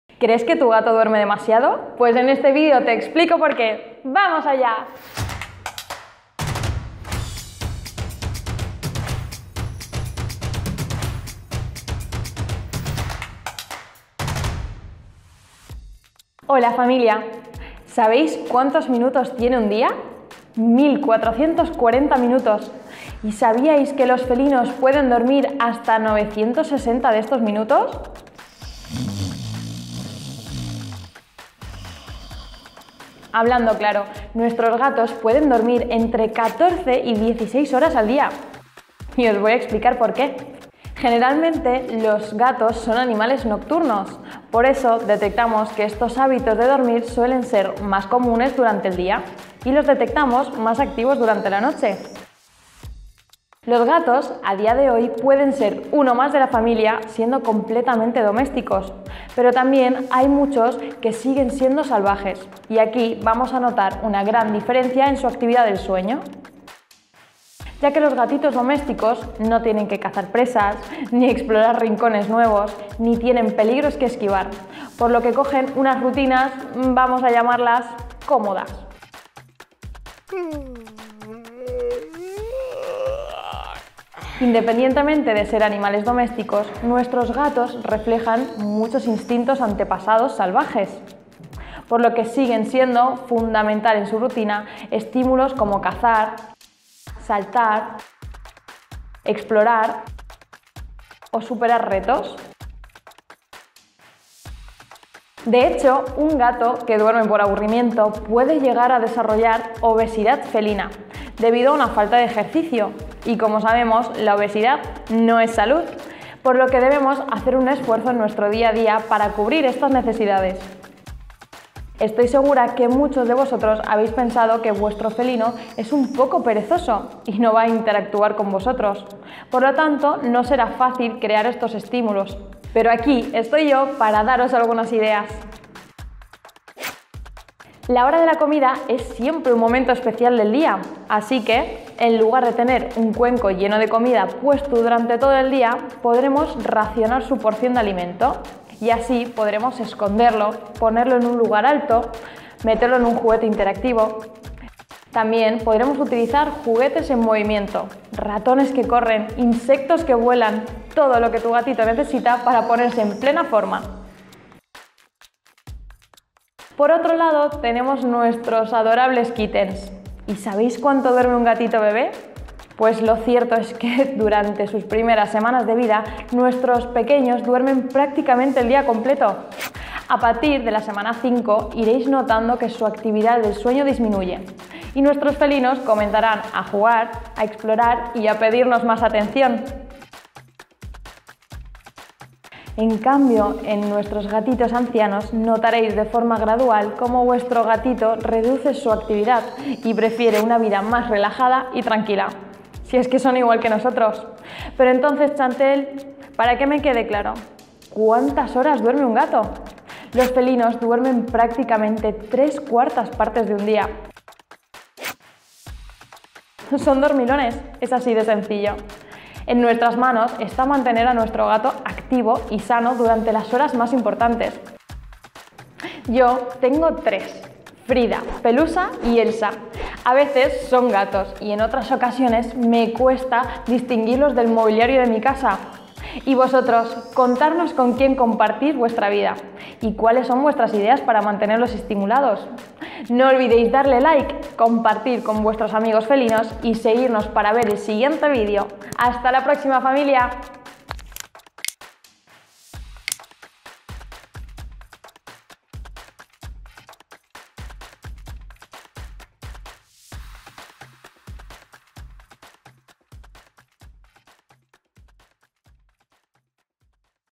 😴 ¿Cuánto DUERME al día un GATO? 🙀
¿Tu gato pasa más de la mitad del día dormido? No te preocupes porque esto es totalmente normal, el único problema que puede haber es que te dé envidia. 😬

𝐈́𝐧𝐝𝐢𝐜𝐞 𝐝𝐞 𝐭𝐞𝐦𝐚𝐬:

00:00 Intro
00:34 Gato adulto
00:40 ¿Por qué duermen tanto?
02:15 Cómo activar a tu gato
02:45 Gato kitten
03:09 Gato senior
03:27 ¿Cuántas horas duerme un gato?

Si tienes alguna duda sobre animales déjanosla en los comentarios y te ayudamos a resolverla

Hecho con amor en Miscota 💙